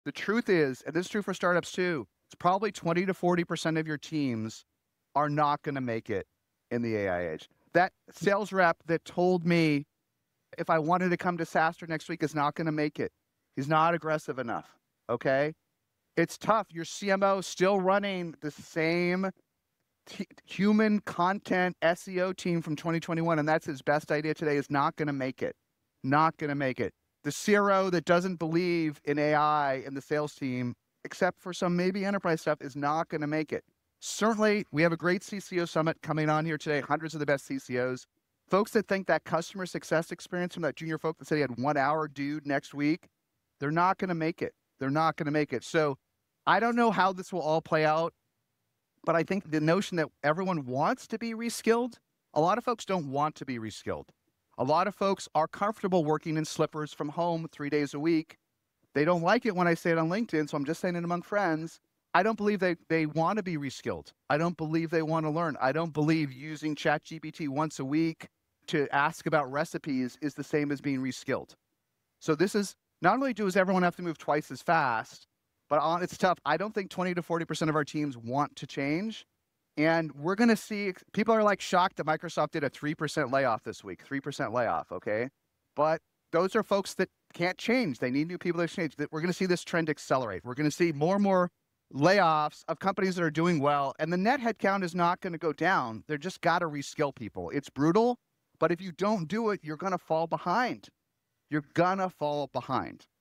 AI Age: 20-40% Job Loss & Reskilling Truth!
In the AI age, we discuss how 20-40% of our teams may not make it and the brutal truth about reskilling. Discover why layoffs will increase even in successful companies.